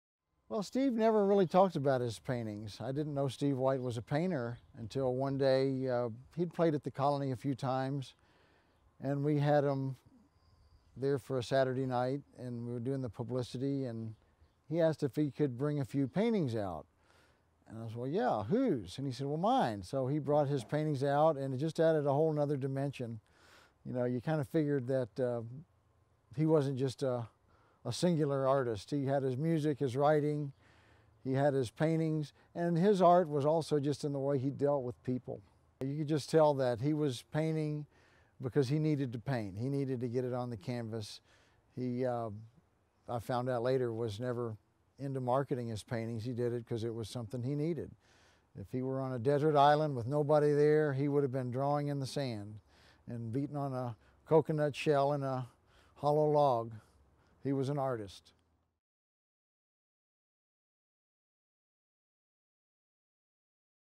Danny Salzhandler raw interview about Steve's paintings
Danny Salzhander in an interesting interview about Steve White and his artistry.